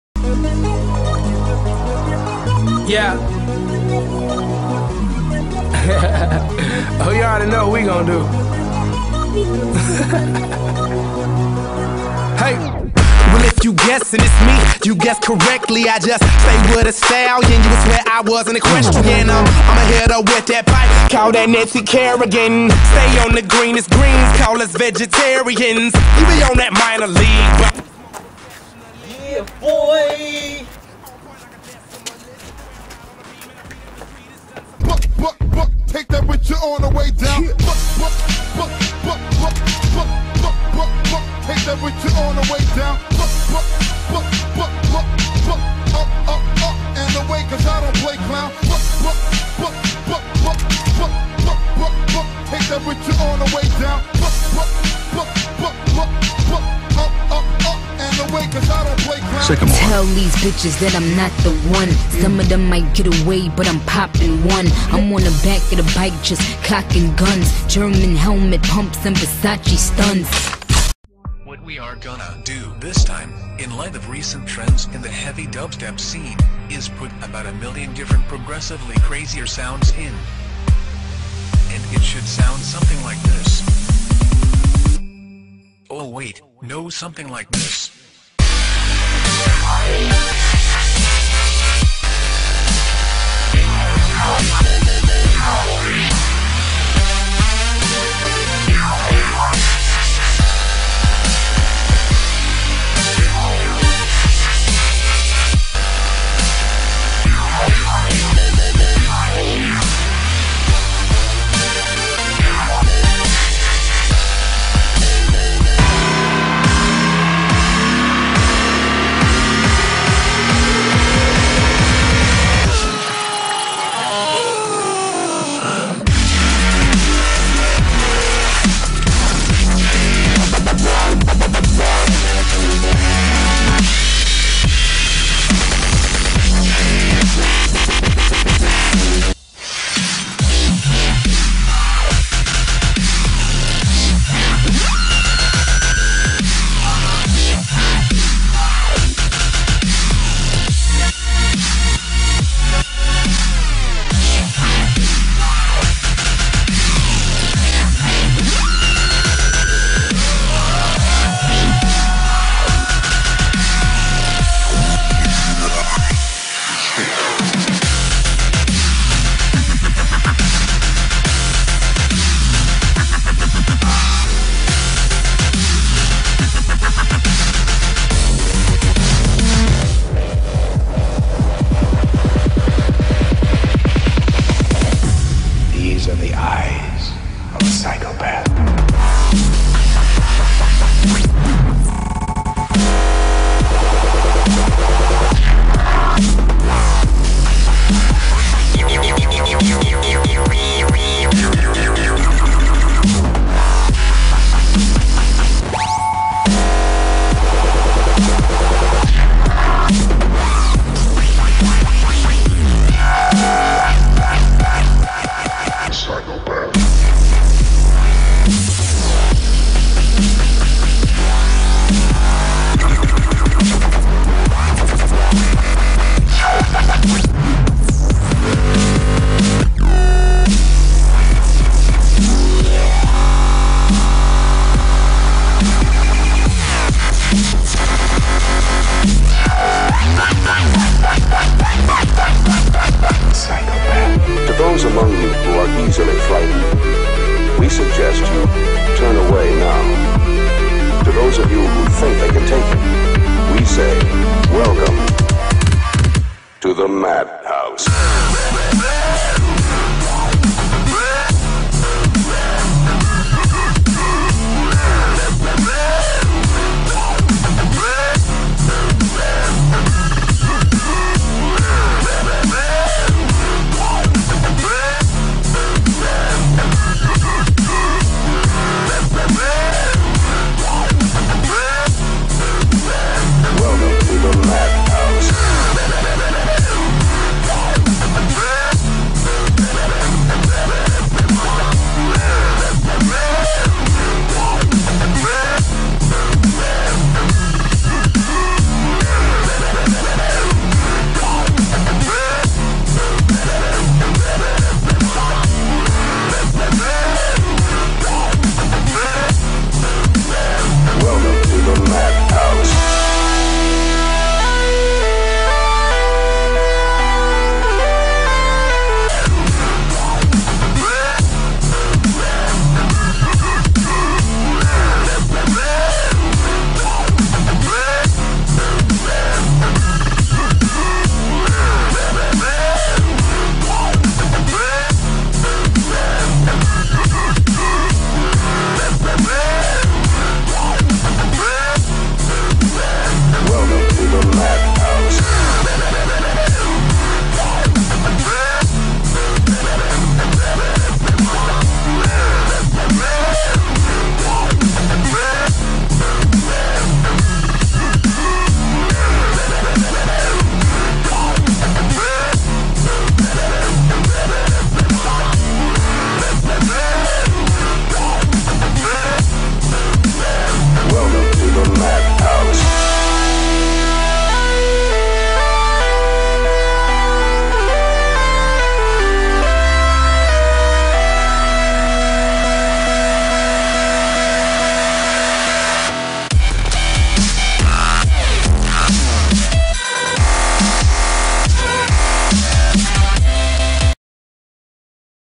- MIX DUBSTEP DANCE *SHUTDOWN & MRKDEANG*
we made the brainbanging stuff in 30 or so min soo bare with us haha it was hard haha trying new concepts and such hope yall enjoy :)